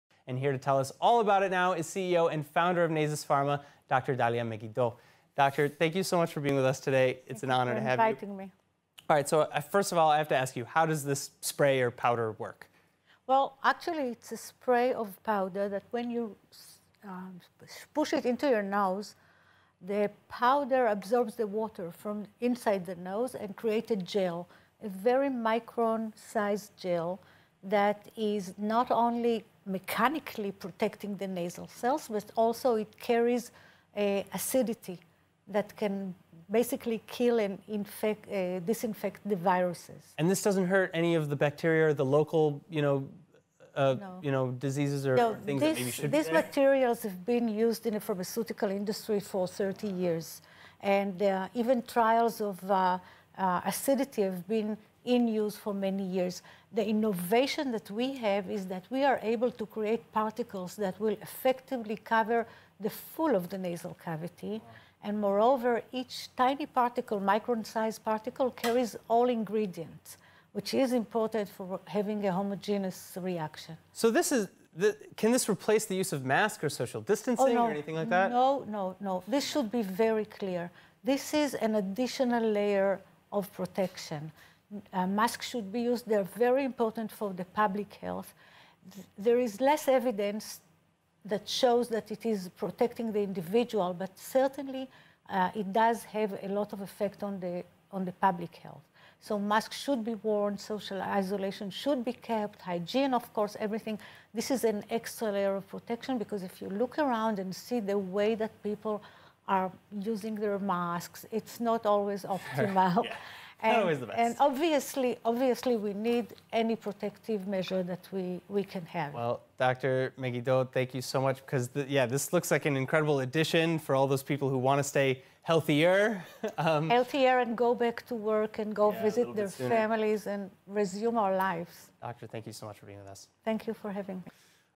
Taffix Nasal Powder Spray Device - Respiratory Protection, ILTV
Nasus Pharma has developed the Taffix™- powder spray device for use as a protective mechanical barrier within the nasal cavity.
Dr Dalia Megiddo on ILTV new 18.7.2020